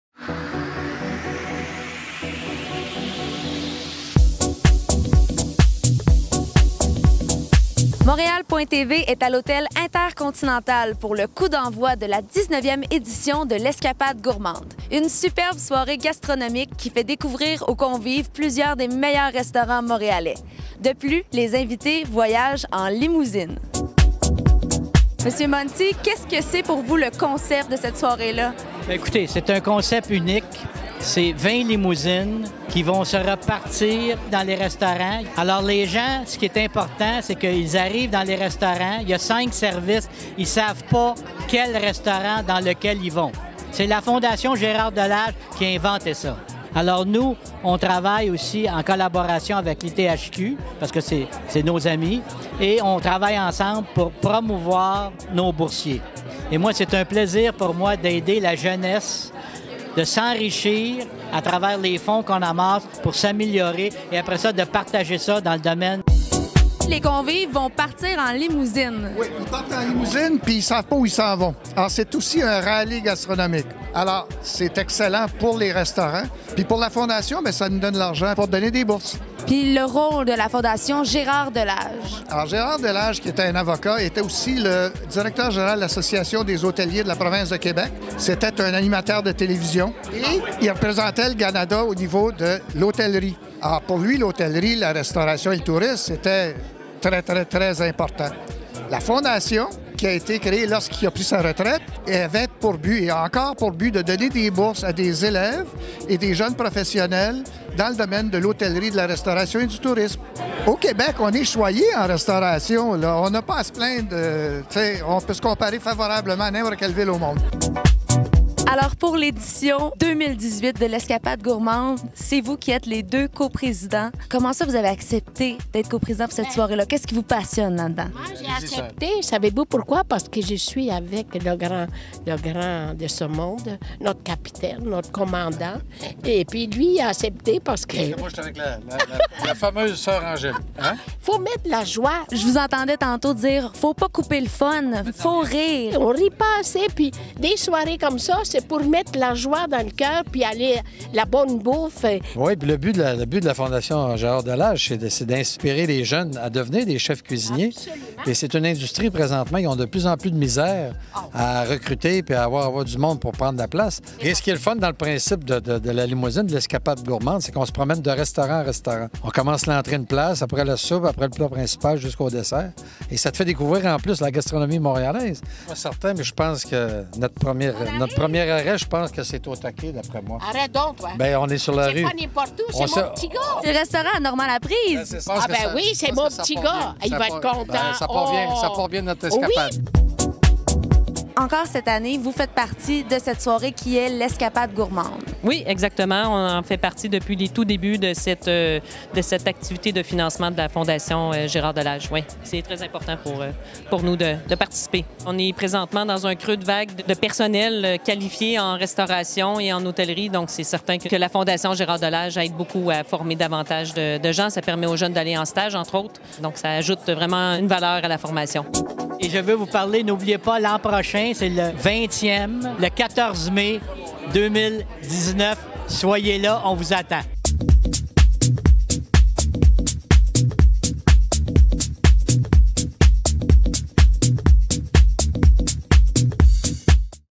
Escapade Gourmande en limousine à Montréal | Montreal.TV
L'Escapade Gourmande, anciennement la Soirée des Patrons, est un événement gastronomique organisé par la Fondation Gérard-Delage.  L'Escapade Gourmande visite en limousine certains des meilleurs restaurants de Montréal.  Dans ce reportage, Pari Monti, Yves Delage, Soeur Angèle, Robert Piché et Christine Lamarche.

Reporter: Carla Bengivengo.